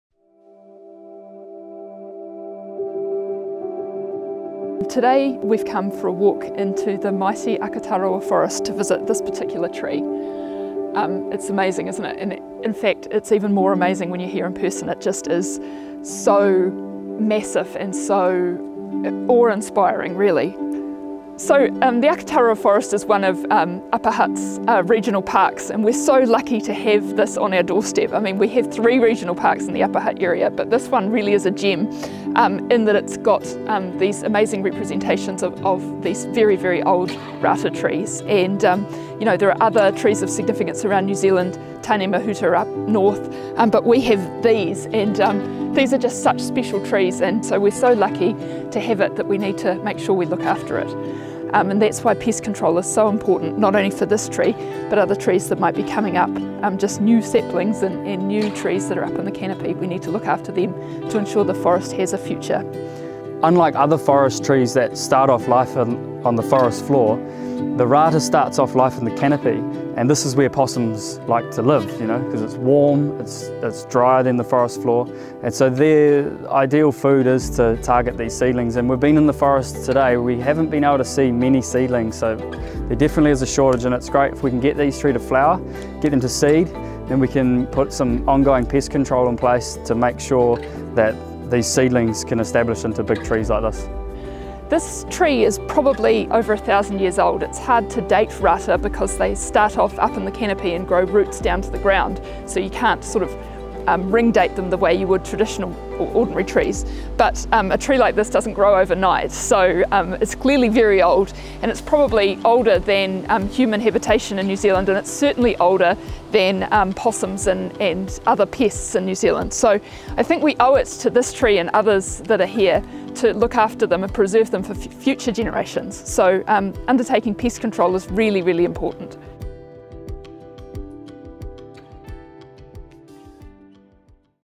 Giant northern rātā in Akatawara forest and the need for pest control
We walked through the old-growth Akatarawa forest to check out the giant northern rātā and to assess the damage being caused by pests.

The Akatarawa forest is one of our regional parks, covering 15,000 hectares of native and exotic forest area between Upper Hutt and the Kapiti Coast.  Much of this forest (12,400ha) is one of our Key Native Ecosystem sites.  These are places of high biodiversity values and it’s an example of original (pre-human) ecosystem types.  We must reduce the possum population to extremely low levels in order to protect these unique forest and wetland ecosystems.